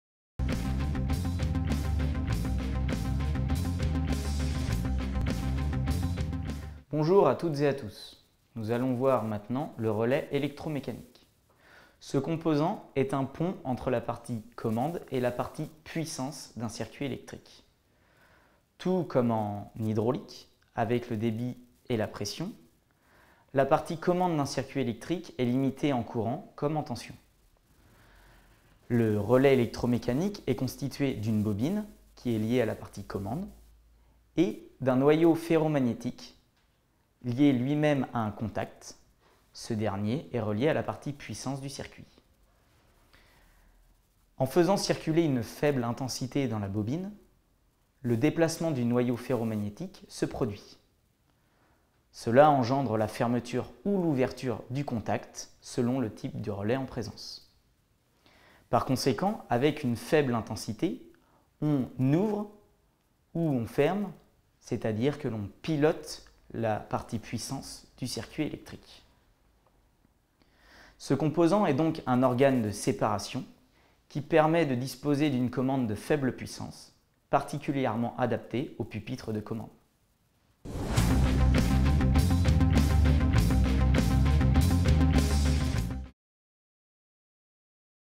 Le relais électromécanique - In Situ Experts Hydrauliciens
Nous allons voir dans cette vidéo “le relais électromécanique”. Ce relais est un composant est un pont entre la partie « commande » et la partie « puissance » d’un circuit électrique.